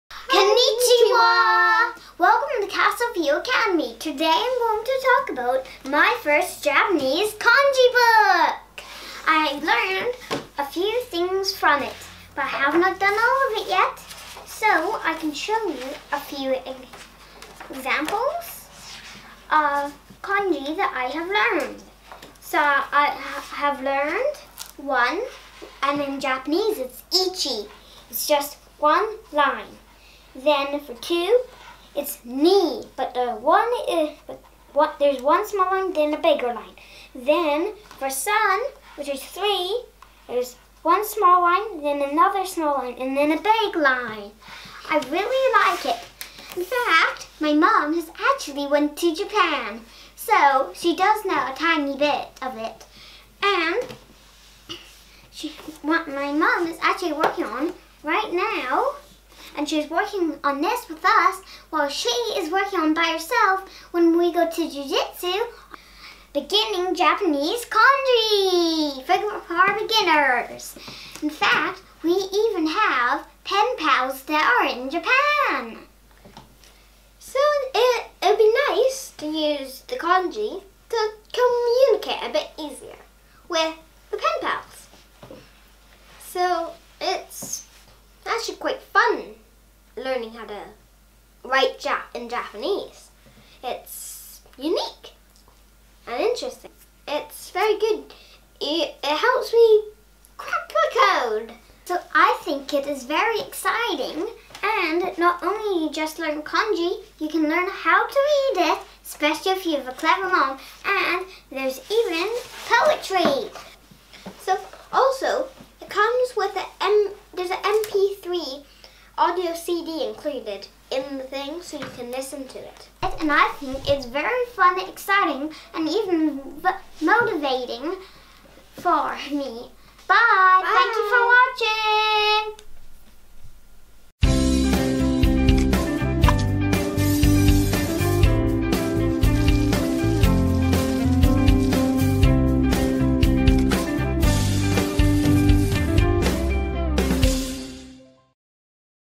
Beginning Kanji and Castle View Academy homeschool
Castle View Academy homeschool reviews Beginning Japanese Kanji and My First Japanese Kanji by Tuttle Publishing.